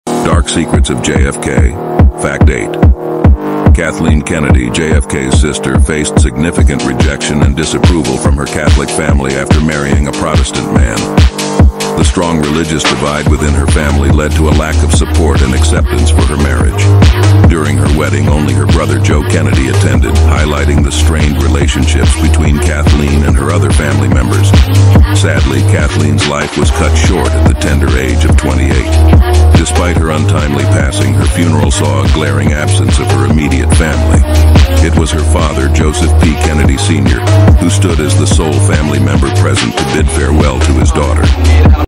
JFK's Dark Secrets: Kathleen Kennedy's Tragic Life
Explore the dark secrets surrounding JFK's sister, Kathleen Kennedy. Witness the significant rejection and disapproval she faced from her Catholic family when she married a Protestant man. Discover the religious divide that led to a lack of support and acceptance for her marriage. Uncover the strained relationships through the absence of her family at her wedding and funeral. Join us as we delve into the tragic life of Kathleen Kennedy, cut short at a young age, and the profound impact it had on her family dynamics.